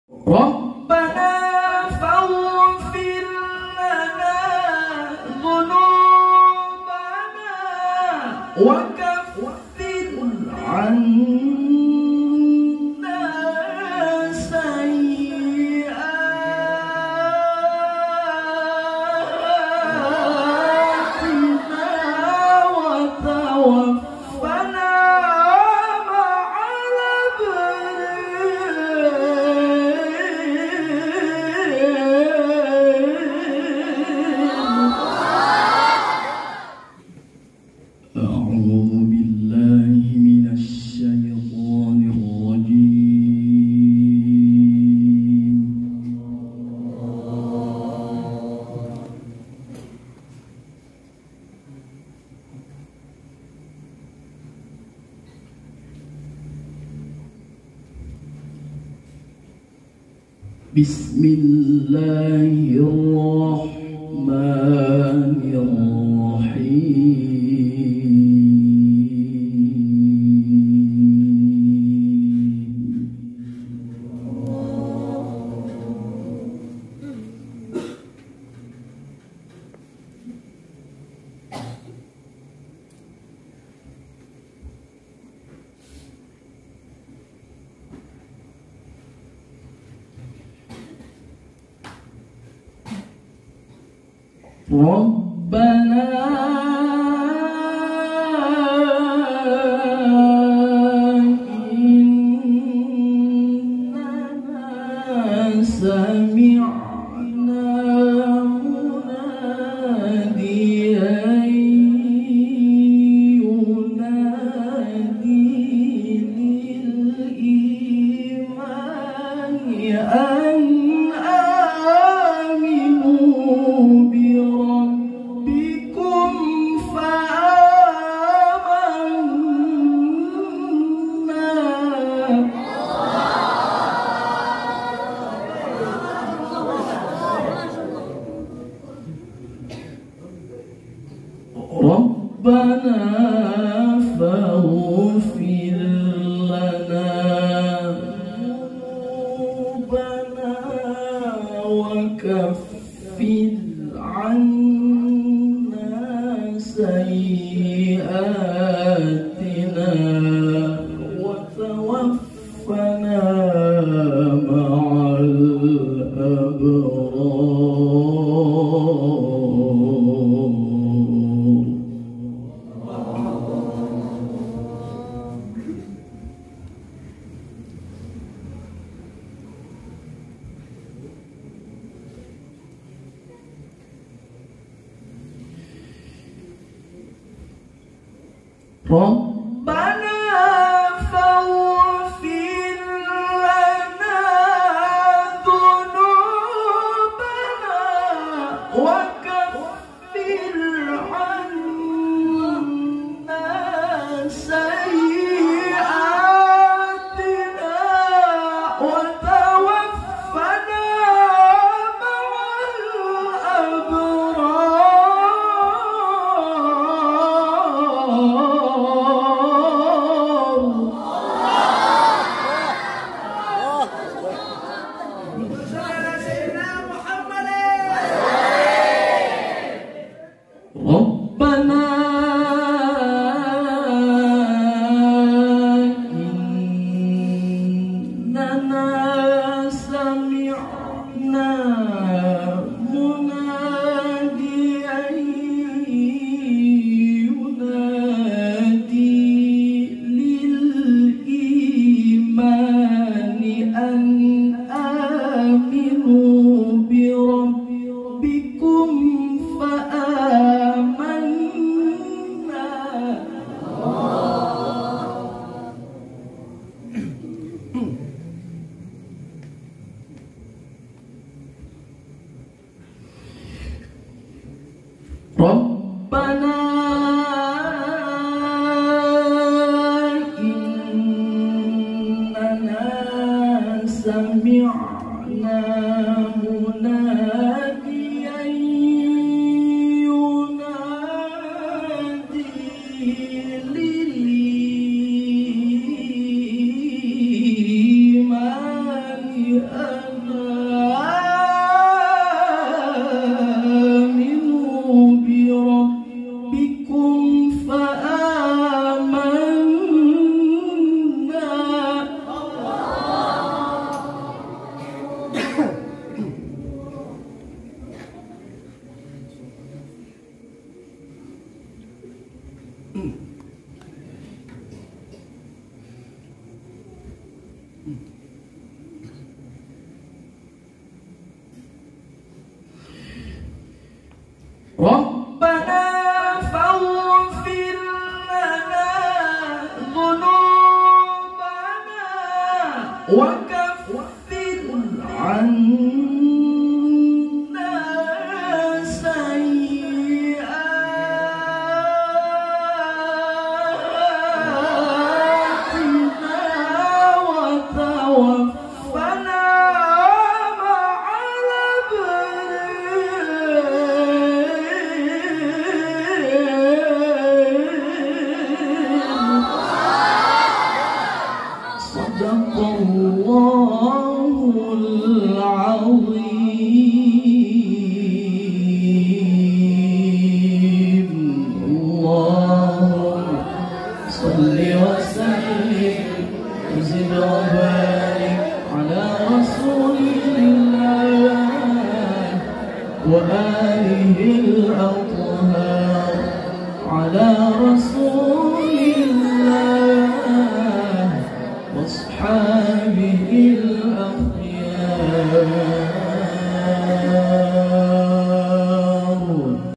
tilawah m. fikri abdul basit pembukaan sanlat mq almumin 2022
tonton sampe selesai semoga berkah amiin.